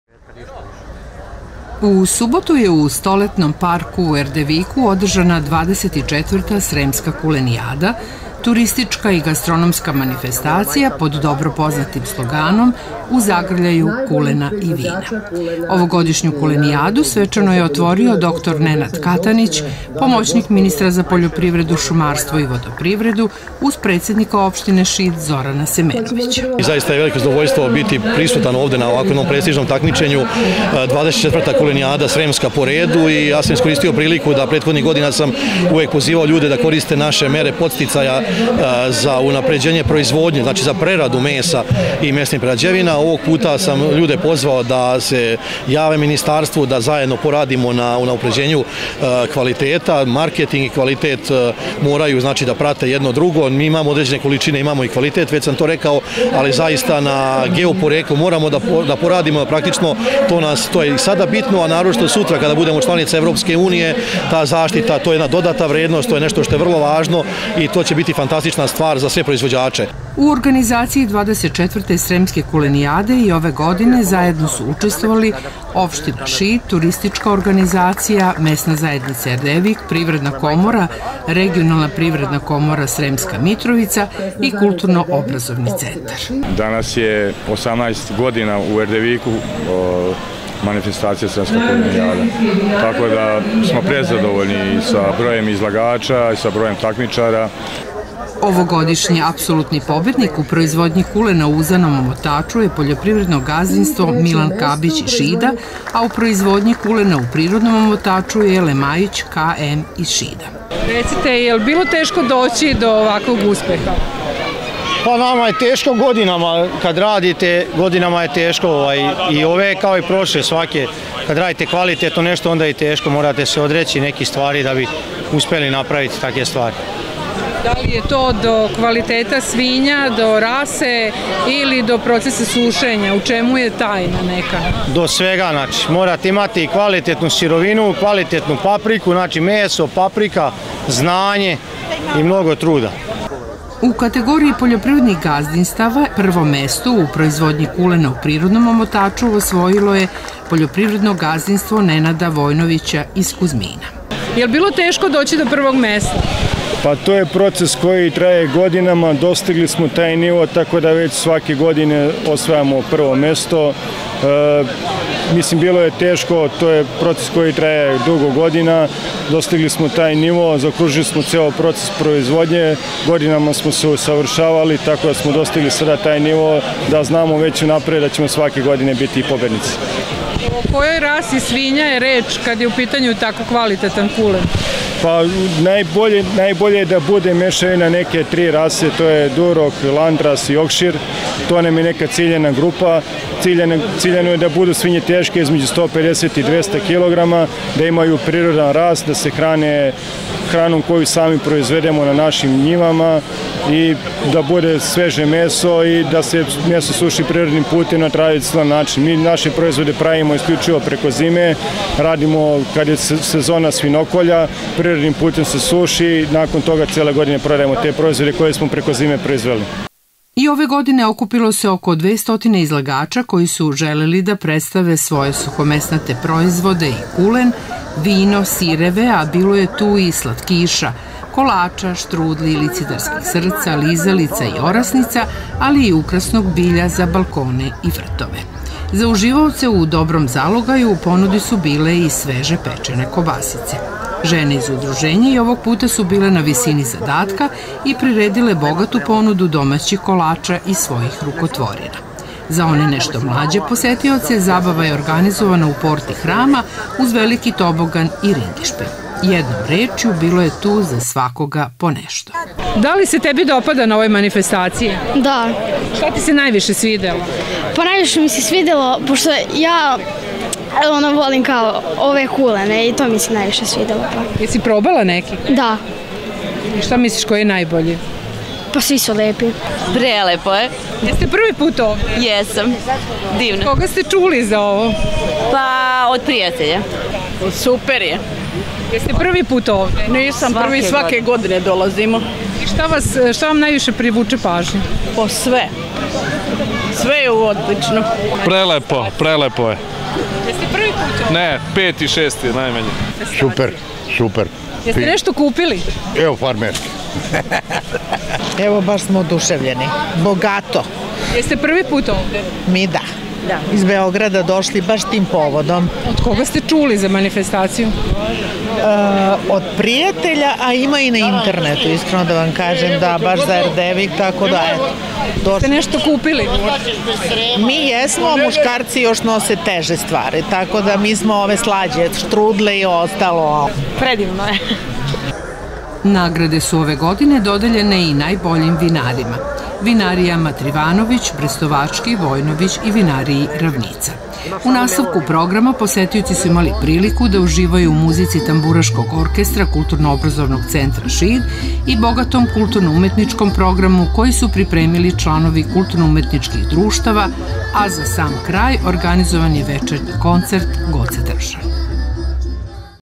24. Sremska kulenijada u Erdeviku
Na ovogodišnjoj 24. Sremskoj kulenijadi koja je u subotu održana u Erdeviku predato je za ocenjivanje kvaliteta ukupno 53 uzorka kulena, 32 u prirodnom i 21 uskom omotaču.Komisija u sastavu dr Milenko Perić, dr Branislav Šojić,  dr Predrag Ikonić i Dragan Vukolić proglasila je apsolutne pobednike u proizvodnji kulena u uzanom omotaču je  PG MILAN KABIĆ iz Šida a  u proizvodnji kulena u prirodnom omotaču je  LEMAIĆ K&M iz Šida.U kategoriji za proizvođače kulena van Srema, pobednik  je preduzetnik Nikola Maksimović iz Valjeva. Ovogodišnju Kulenijadu svečano je otvorio dr Nenad Katanić, pomoćnik ministra  za poljoprivredu, šumarstvo i vodoprivredu.